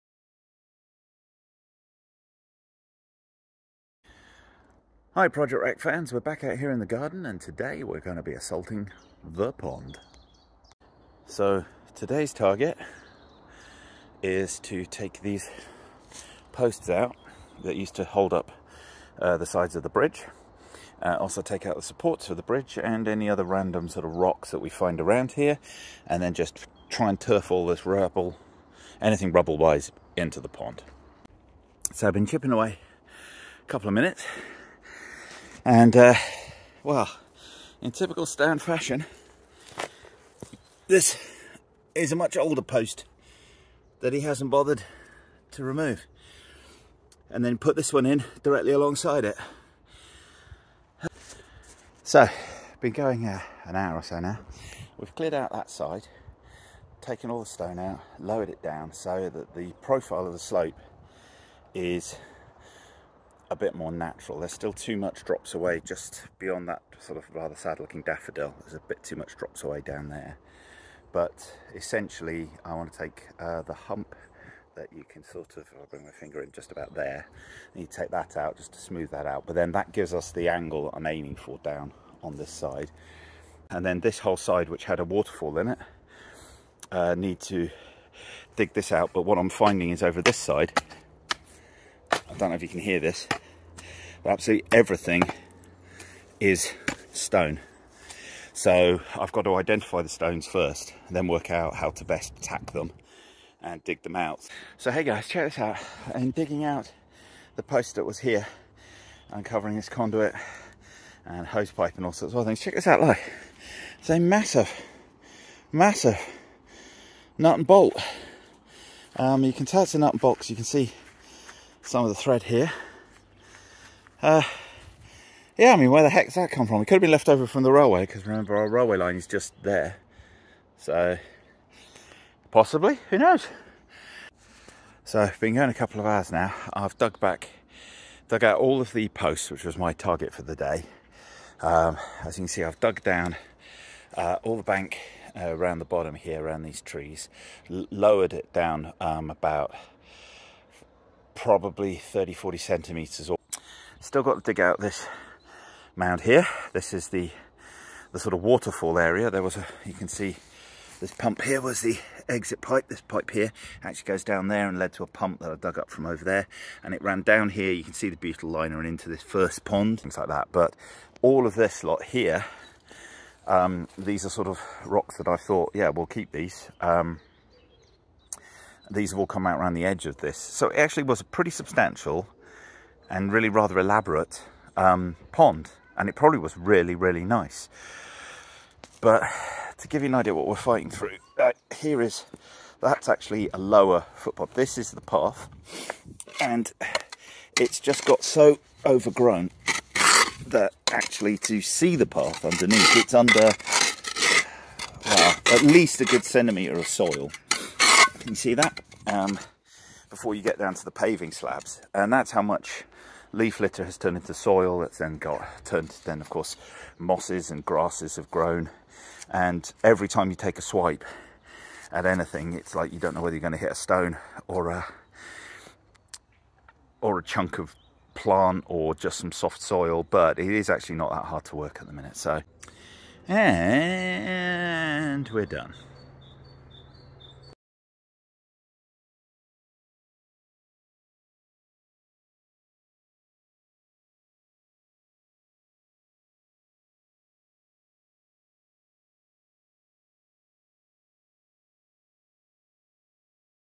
Project Wreck: The Pond!
Project Wreck : The Pond - The final part of the Pond filling in project, which ultimately came about because I needed to demolish the patio wall.